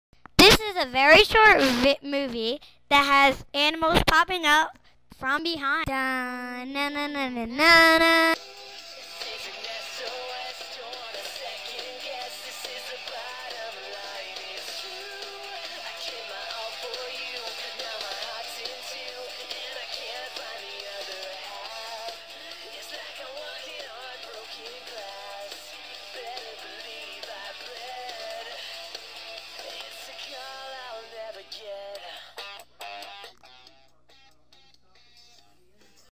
Popping
A short video of animal popping out from behind a bench